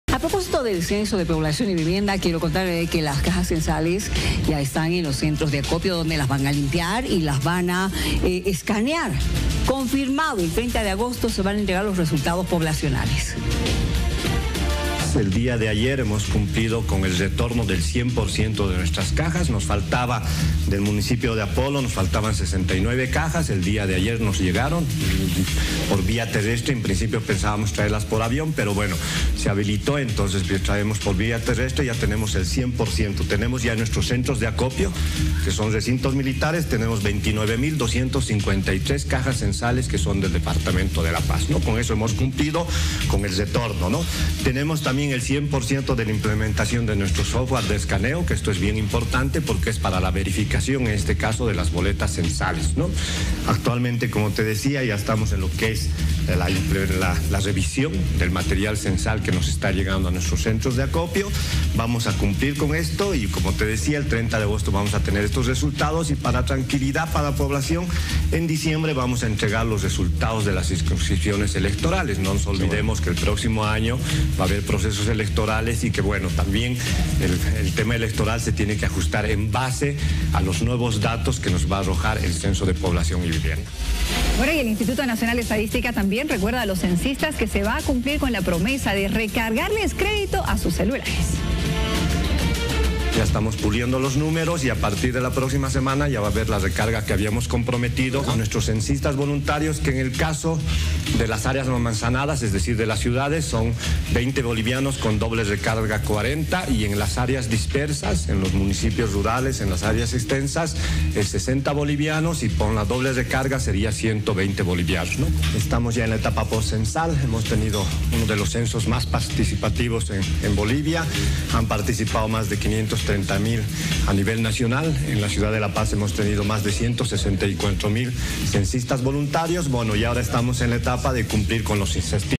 Censo 2024: Realizarán el escaneo de cartillas
Las cajas censales se encuentran en los centros de acopio y se confirmó la fecha en la que se brindarán los datos obtenidos.

Conversamos con el responsable departamental del Instituto Nacional de Estadística, Santiago Padilla, que nos brinda más detalles de la etapa post censal que incluye la entrega de los incentivos a los voluntarios.